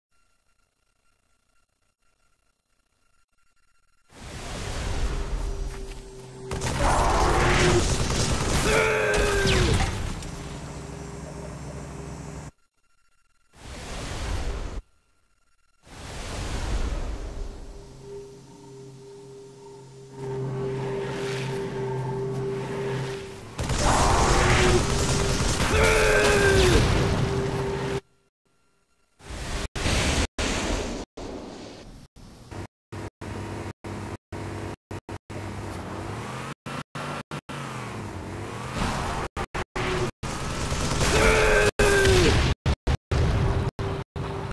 So Owned
The Bubble Shield isnt helping him,  my friend pride is saved by a warthog from the heavens